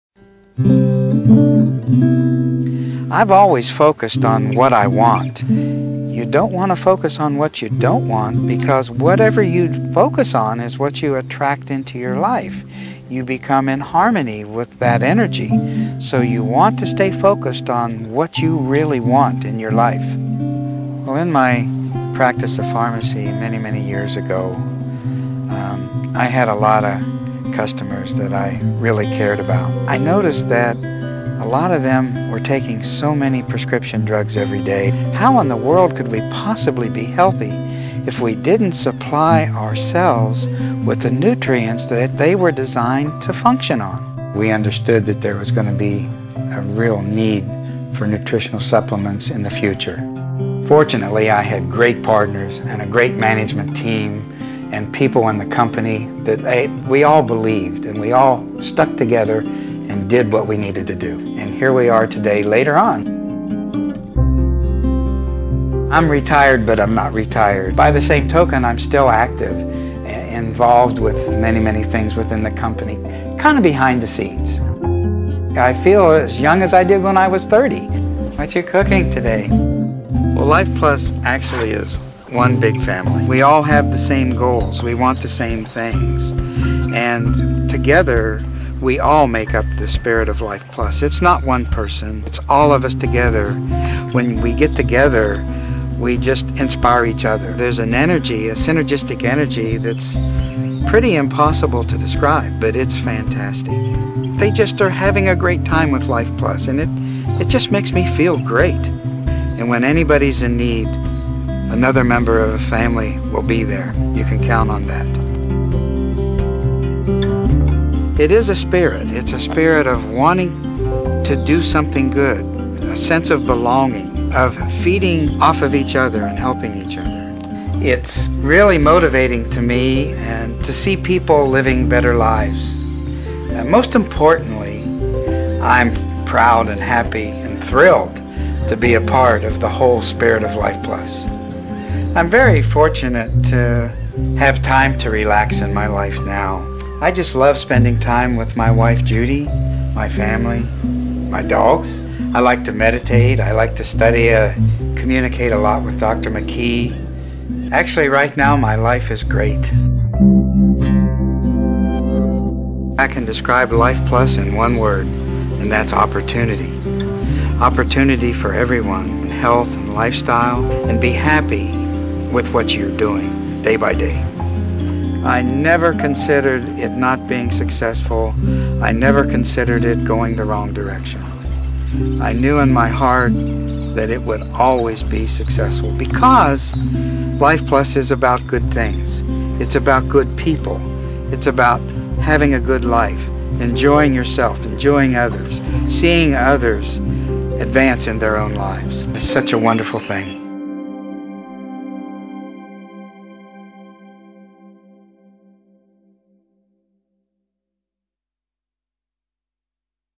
Bob Lemon über Lifeplus
In diesem Film spricht einer unserer Gründer, Bob Lemon, über die Vision und Philosophie, die zur Gründung von Lifeplus führten.

NEHMEN WIR VERBINDUNG AUF